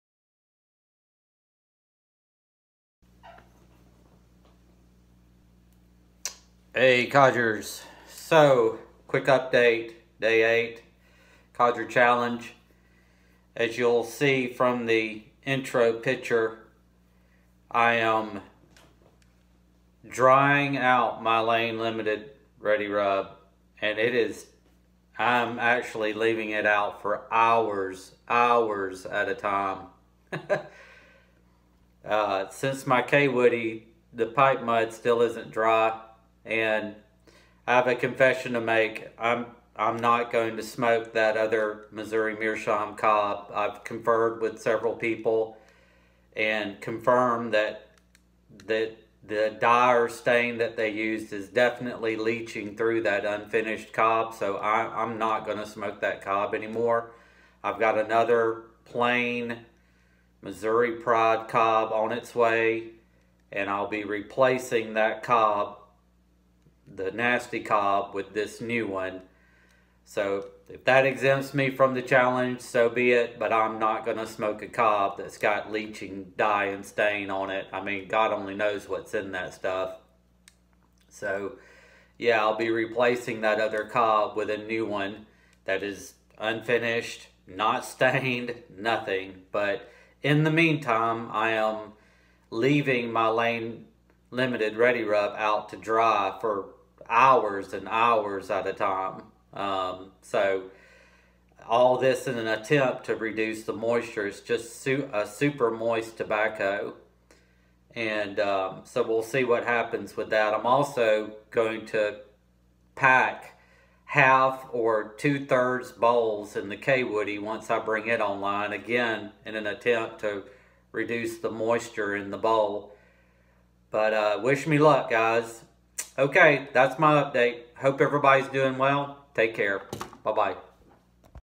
Codger Challenge 2021 - Day 8 - EXTRA LONG DRY TIME & NEW COB
Take note of the # of pipe cleaners in the pic!! 😱😳  Figuring out that LRR is a tobacco best smoked in a small bowl (at least right out of the tub) ...  Have left ounces out to air dry for hours